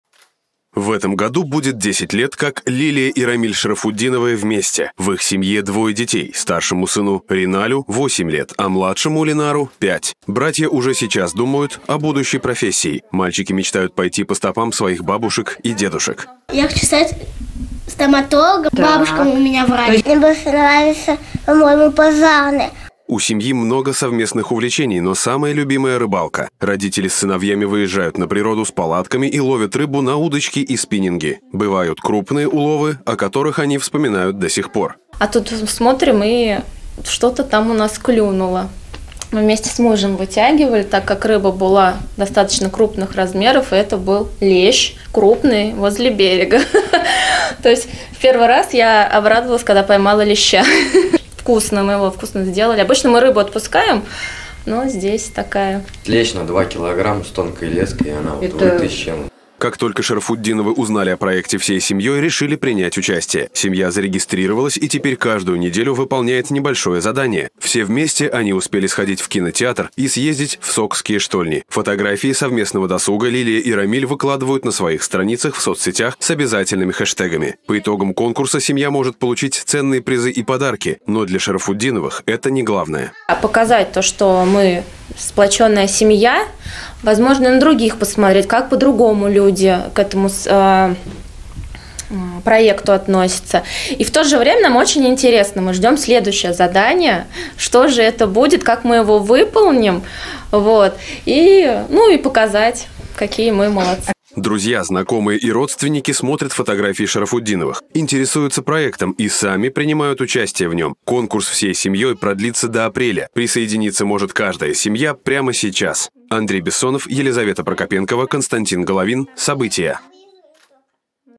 Самарцев приглашают принять участие во Всероссийском проекте "Всей семьей"
В России стартовал большой проект "Всей семьей". Он приурочен к объявленному президентом Году семьи и направлен на укрепление традиционных семейных ценностей. К проекту присоединились и самарские семьи. С одной из них познакомились наши корреспонденты. Как участники проводят время всей семьей, расскажем в сюжете.